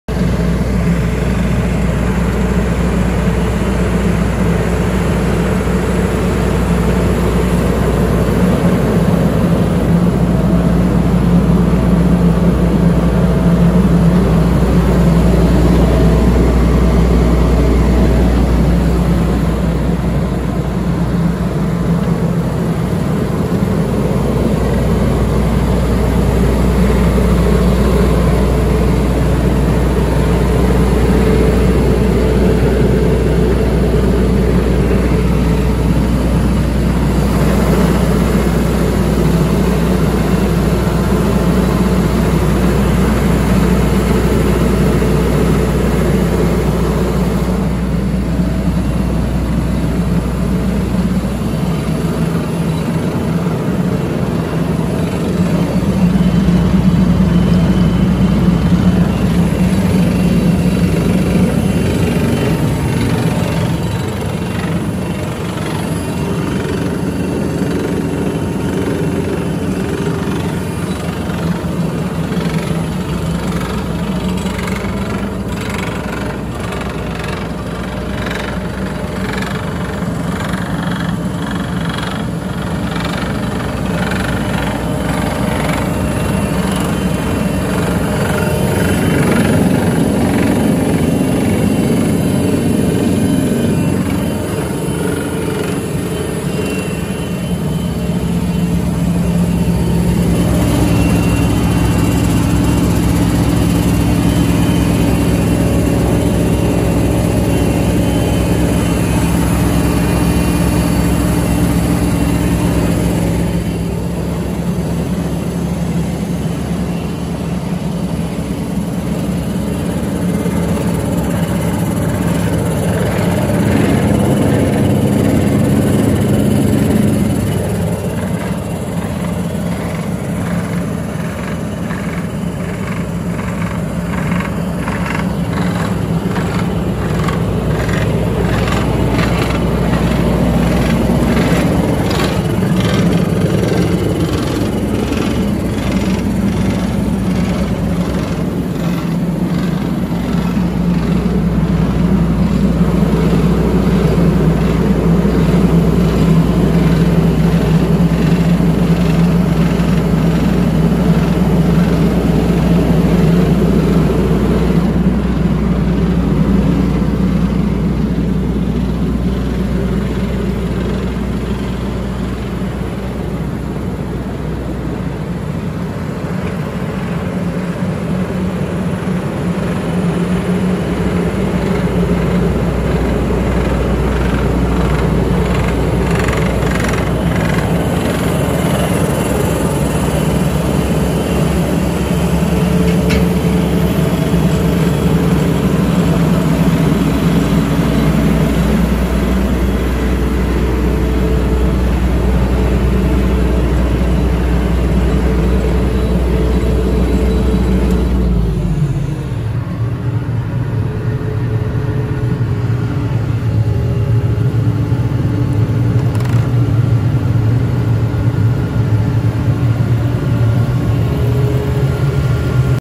Vogele S 1600-3 -2013- MAC 2000 srl
Brand: Vogele
Model: S 1600-2
Year: 2013
Serial number: 11823794
Hours: 7.470
Weight: 18,4 Ton
Screed: AB 500-2 TV
Leveling system: n.2 sonic sensor  / n.2 sonic auger sensor
Optional: automatic screed adjustment / n.2 extentions of 0,25m each